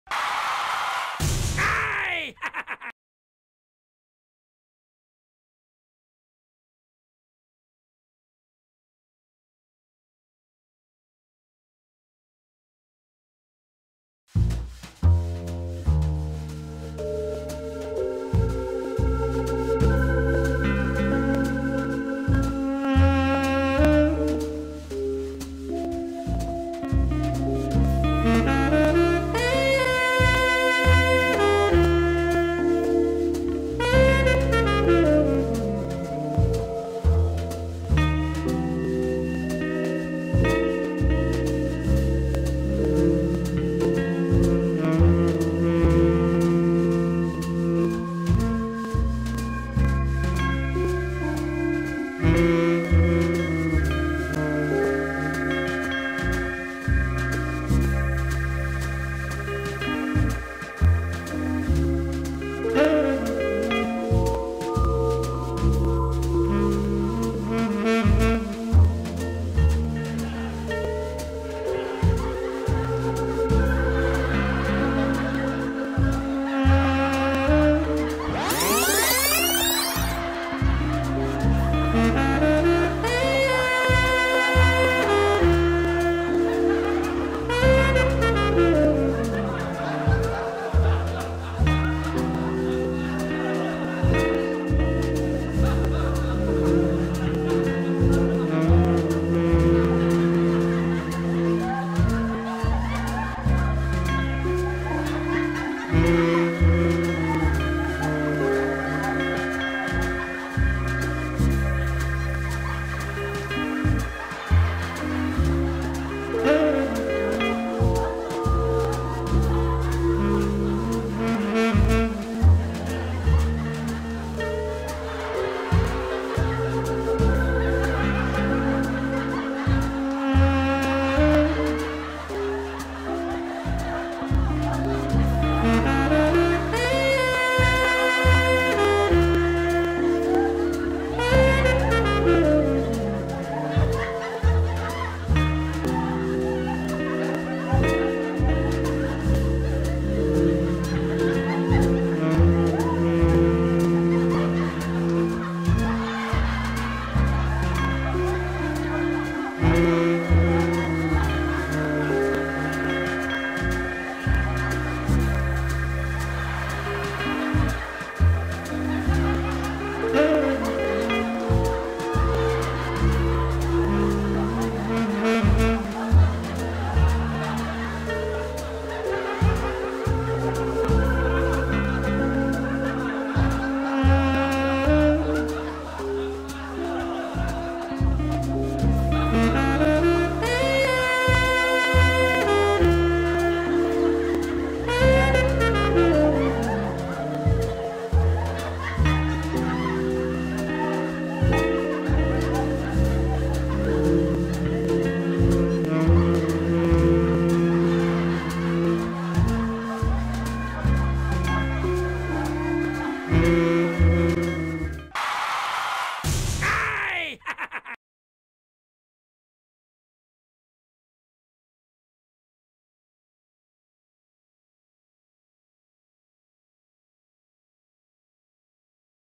Accidentally sitting on a baby PRANK
A distracted man on a phone accidentally sits on a baby